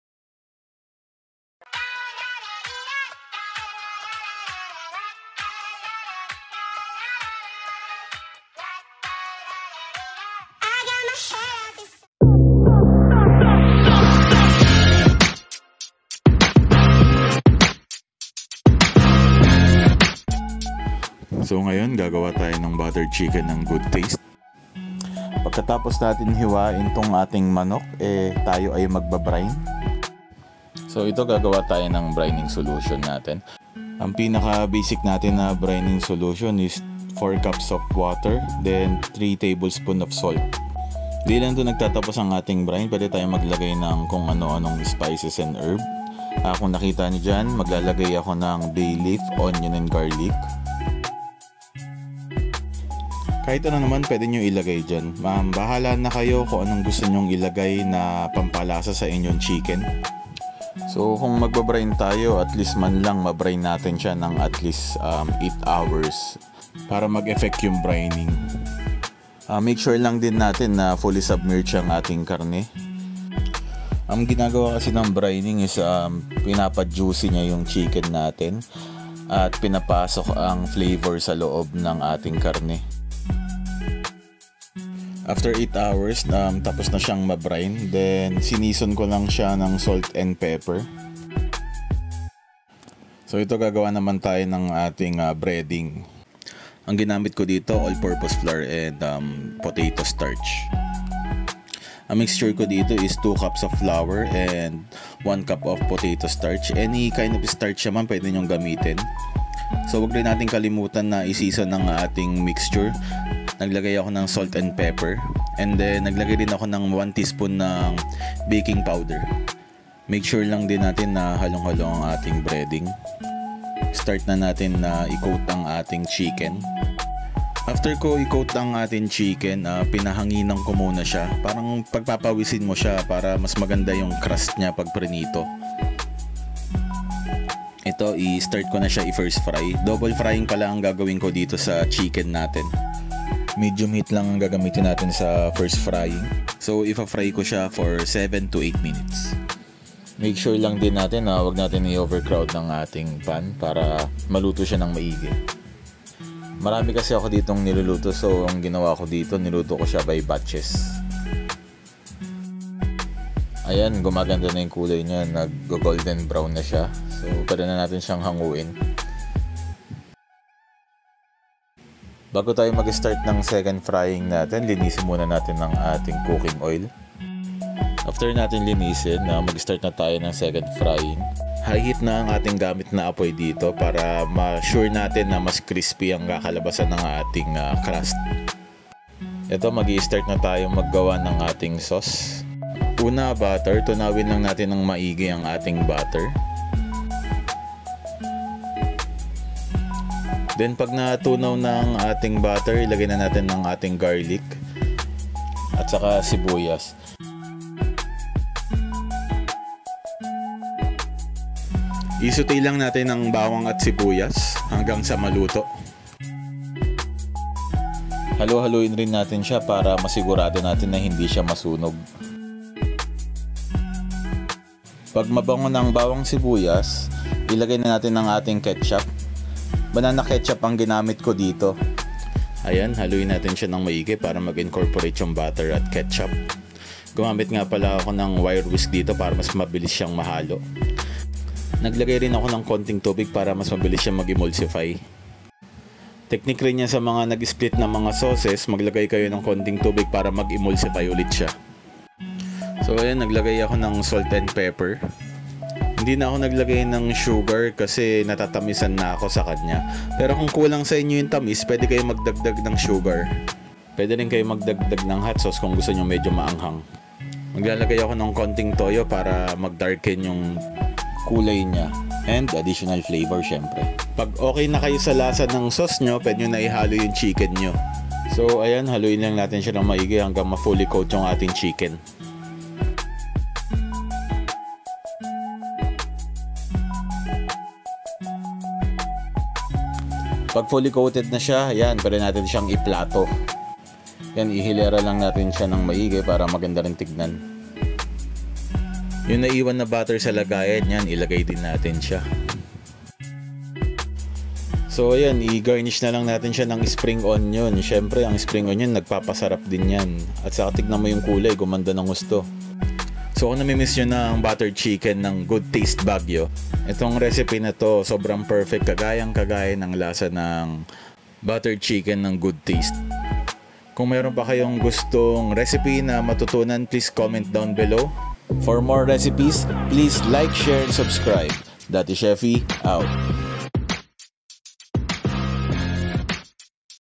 BUTTERED CHICKEN ala "Goodtaste Baguio"
INGREDIENTS

chicken  any part  1kg
season with salt and flour

BREADING-
all purpose flour 2cups
potato starch/cornstarch 1cup
salt 1tbsp
pepper 1tsp

SAUCE -
butter 100 grams or 1/4 cup
banana ketchup 1/2 cup